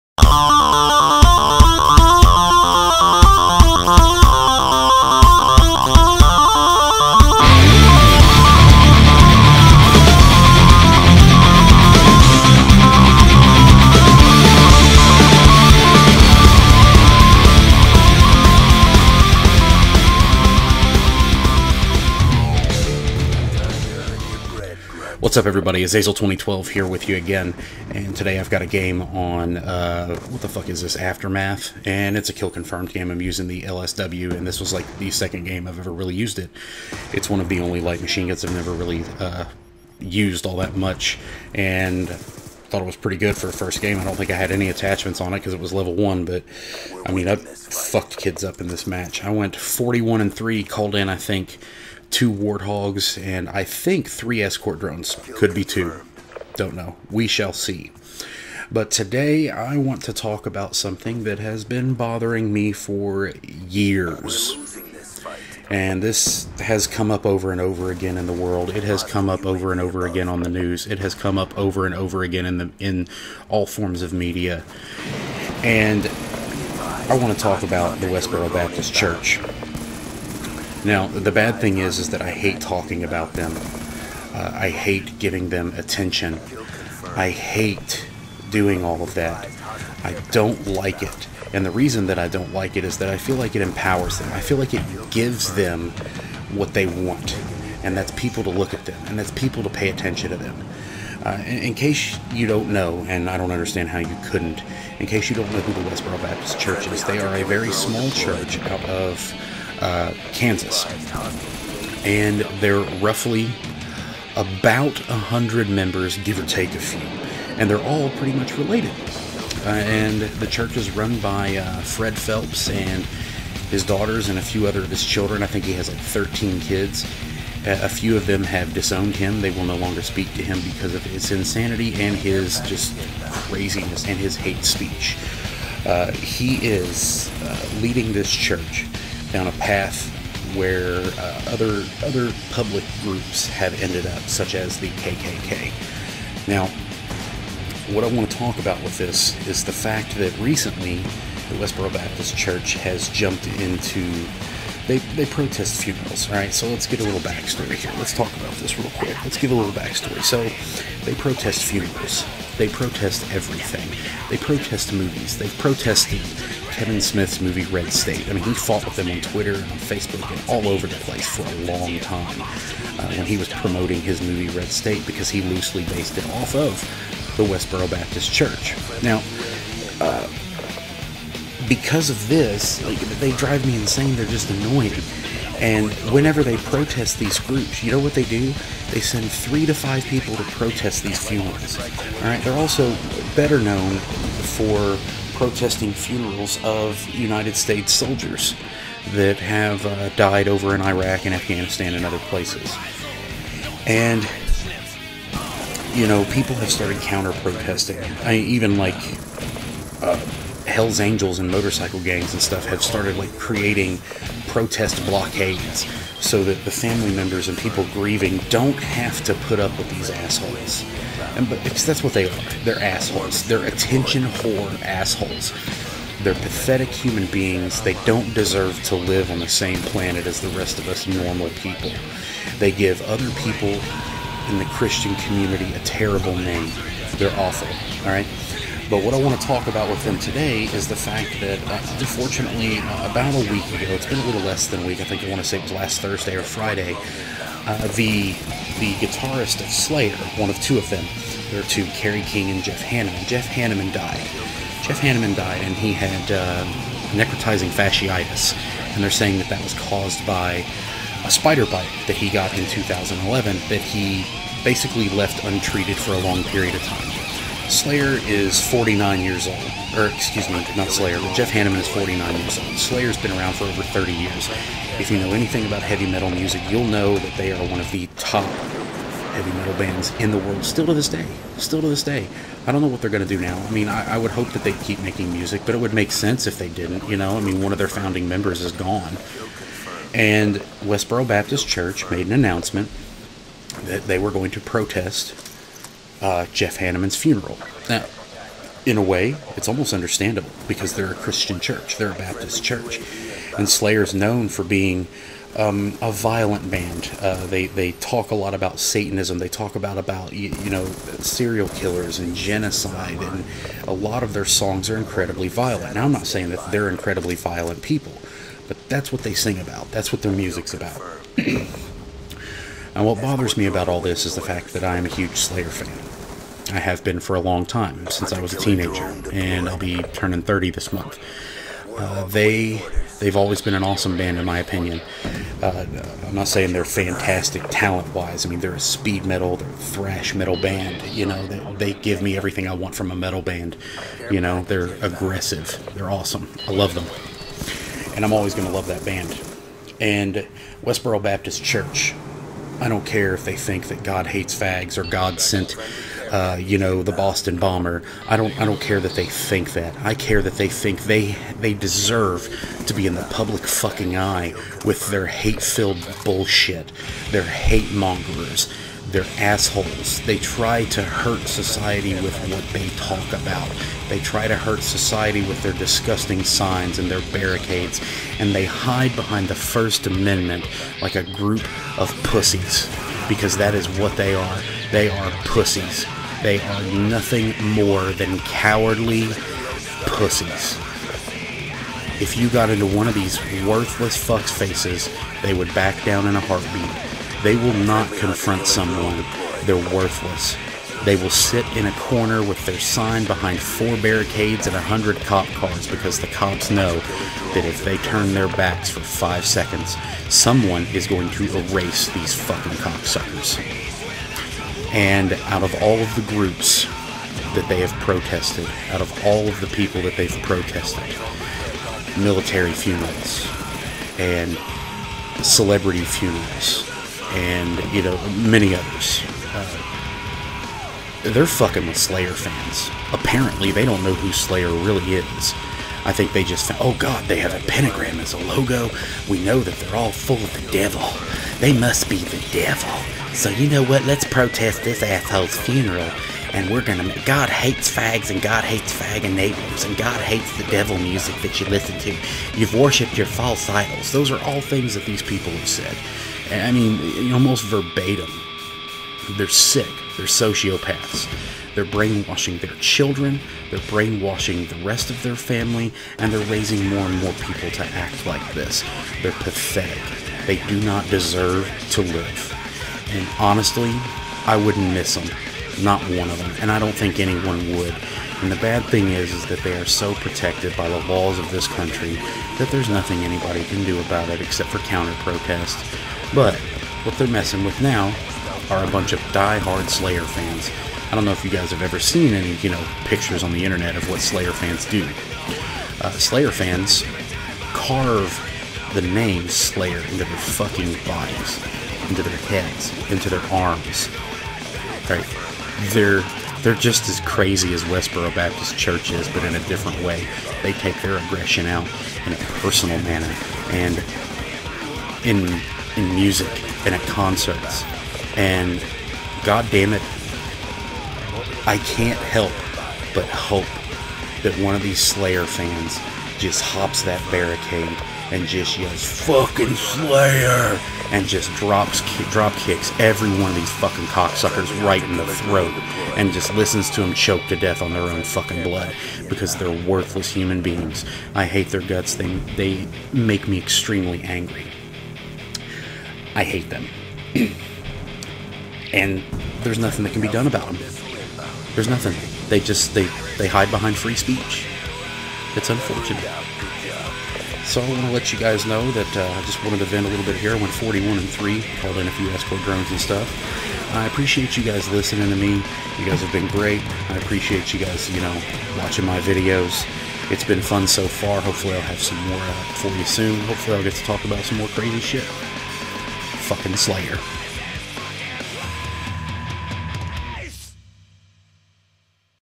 Fuckin' Slayer and Fuckin' Westboro Baptist Church
A pretty awesome Kill Confirmed game on the terrible map Aftermath.  I go 41-3 and my Scorestreaks do some work.  My topic today is on the Westboro Baptist Church, and how they're going to protest the funeral of Slayer's guitarist Jeff Hanneman.  Yeah...let's go ahead and find out how a bunch of bloodthirsty metalheads deal with Westboro's shit!  FUCKIN' SLAYERRRRRRR!!!